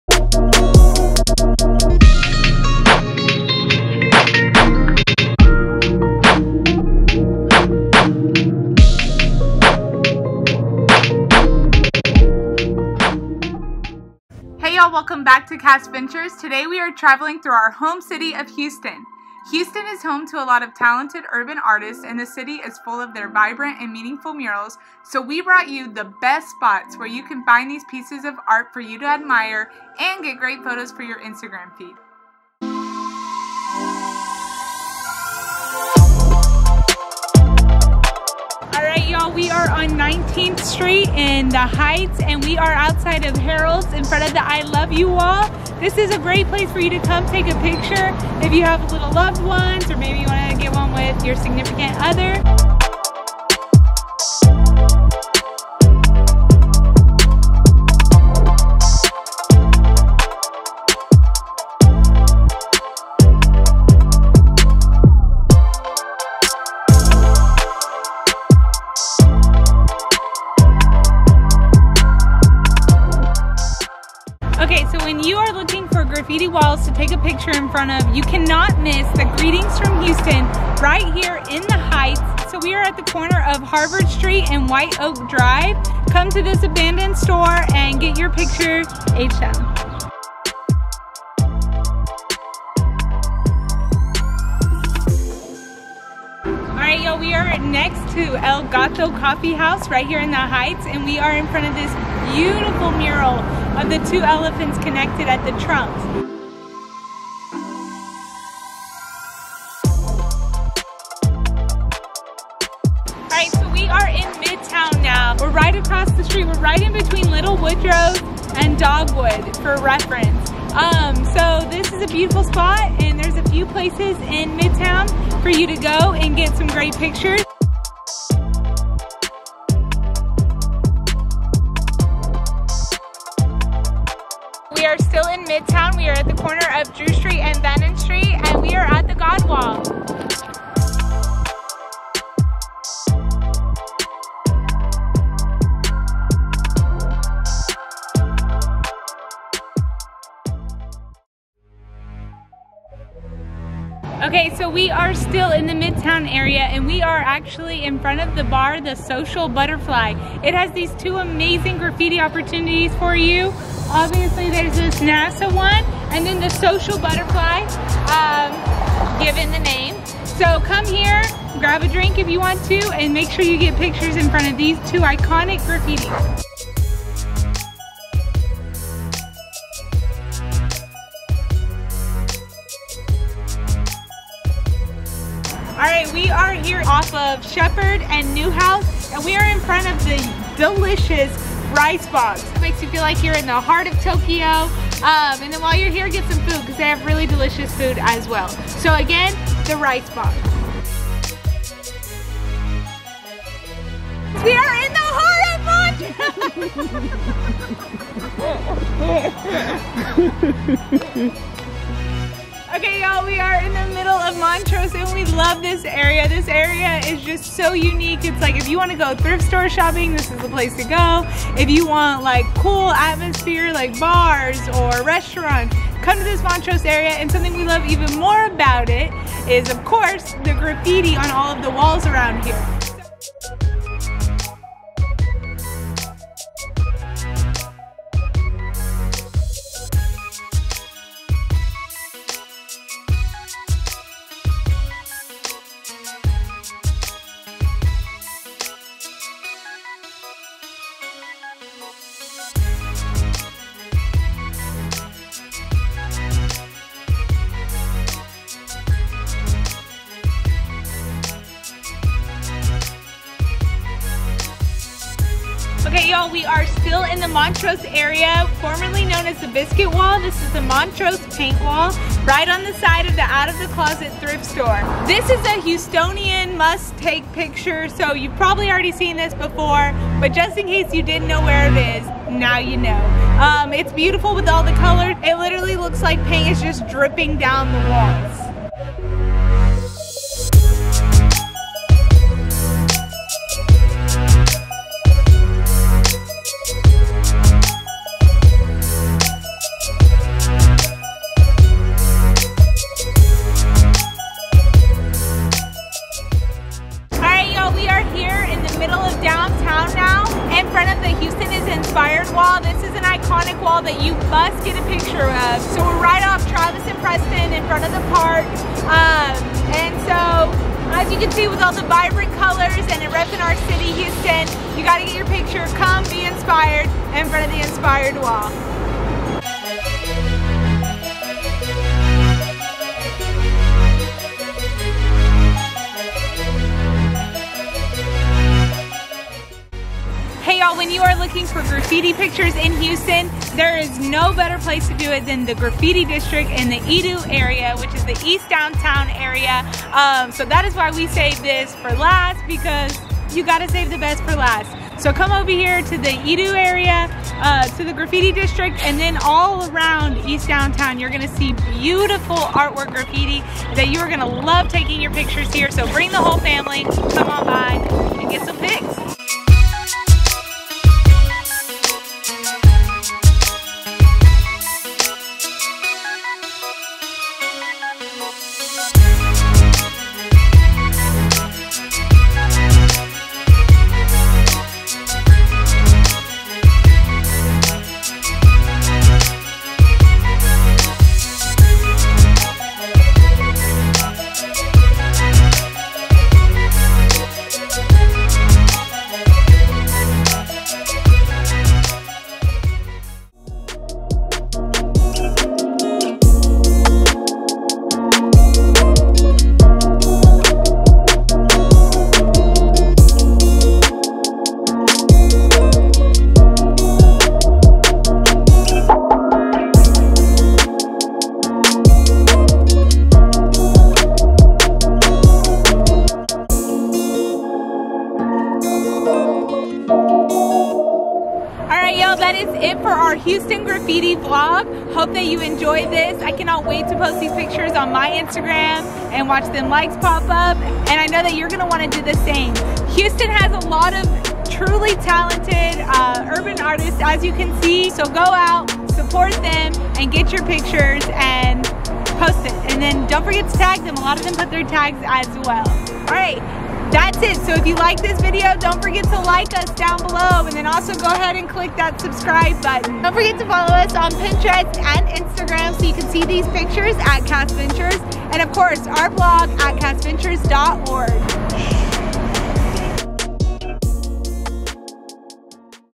THE BEST OF HOUSTON'S URBAN ART
Travel from home like we are. this summer we are traveling from home and visiting the tourist attractions the we would normally do in other cities, but we are doing them in our home town. On this vlog, we are taking you to some of Houstons best Art Walls. Houston has TONS of talented individuals and some of them express their art in forms of graffiti or art murals. Check out this video and see for yourself what we are talking about. Enjoy!